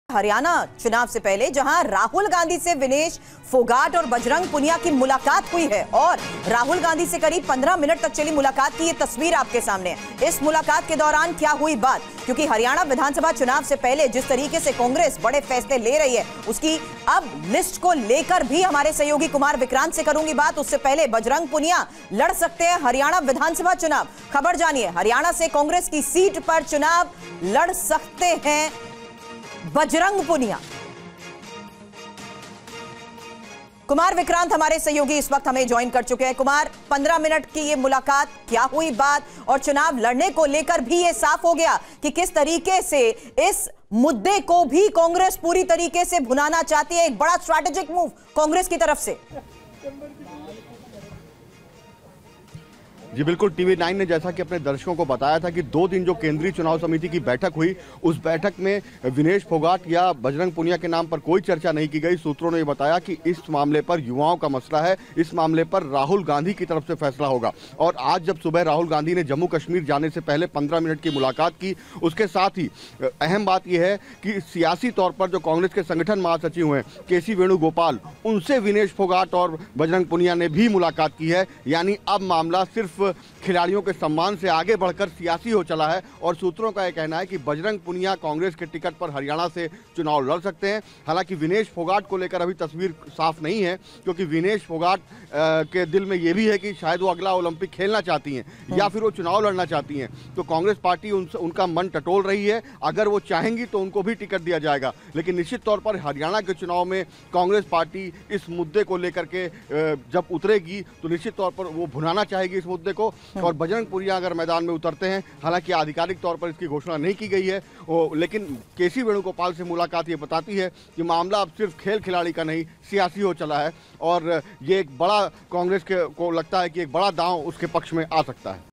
Haryana Election: राहुल से विनेश-बजरंग की मुलाकात... Congress के टिकट पर चुनाव लड़ेंगी Vinesh phogat
Haryana Election: Rahul Gandhi से विनेश-बजरंग की मुलाकात... Congress के टिकट पर चुनाव लड़ेंगी Vinesh Phogat